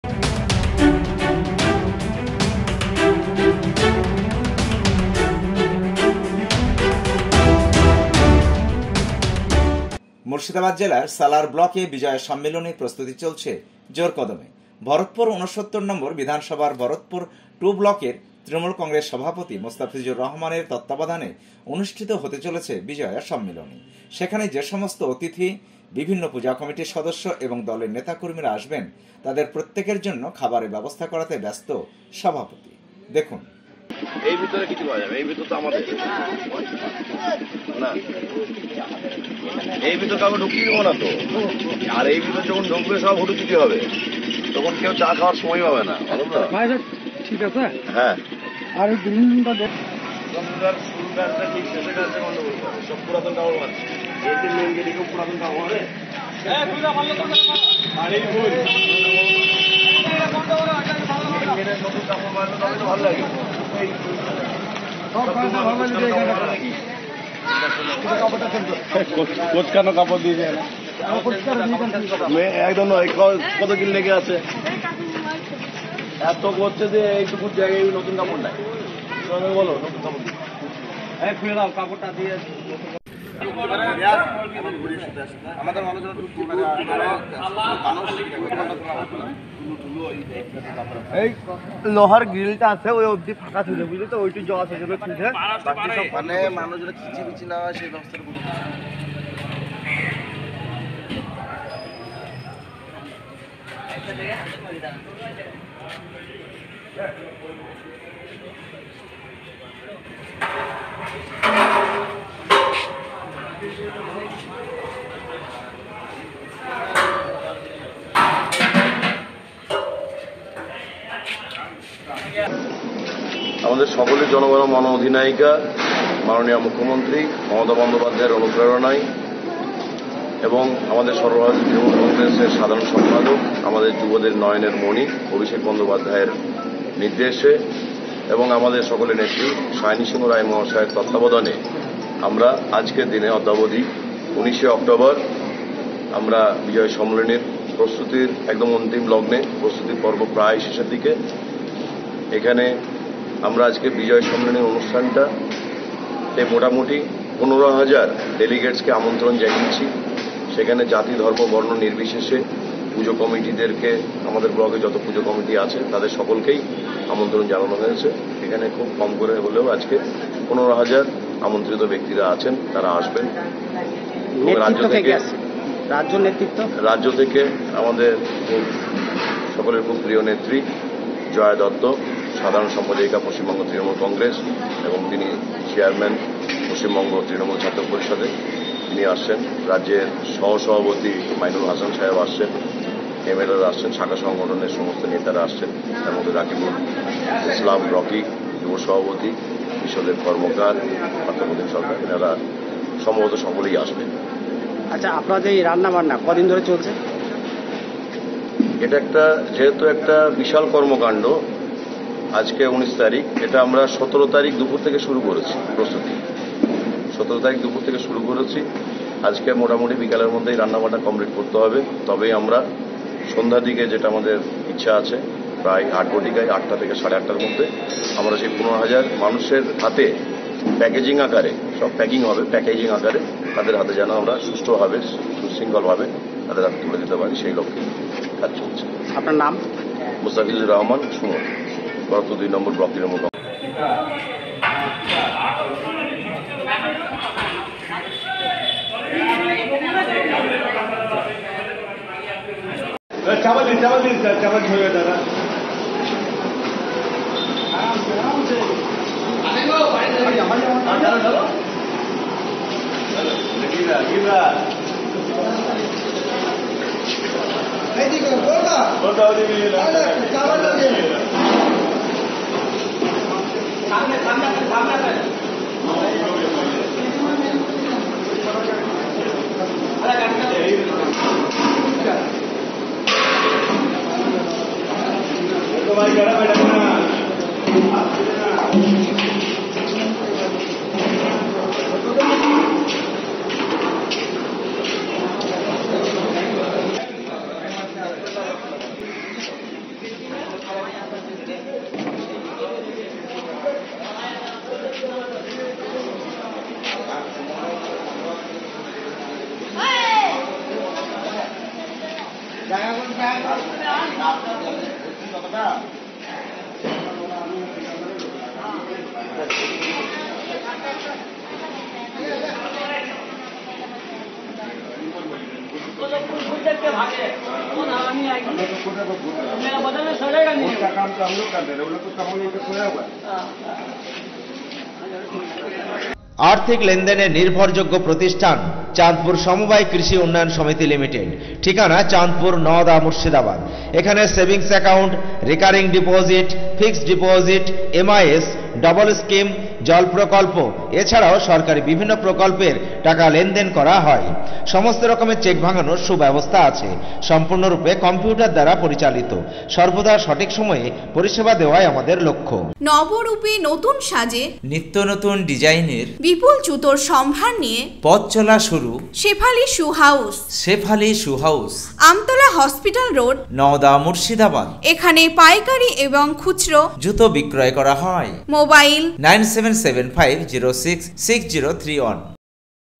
ভরতপুর ২ ব্লক TMC সভাপতি।।মোস্তাফিজুর রহমানের নেতৃত্বে।।বিজয়া সম্মিলনীর প্রস্তুতি চলছে জোর কদমে।।
BHARATPUR TWO BLOCK TRINAMOOL CONGRESS PRESIDENT MOSTAFIJUR RAHAMAN LED THE PREPARATION FOR VIJAYA S  ভরতপুর ২ ব্লকের তৃনমূল কংগ্রেস সভাপতি।। মোস্তাফিজুর রহমানের নেতৃত্বে।।বিজয়া সম্মিলনীর প্রস্তুতি চলছে জোর কদমে।।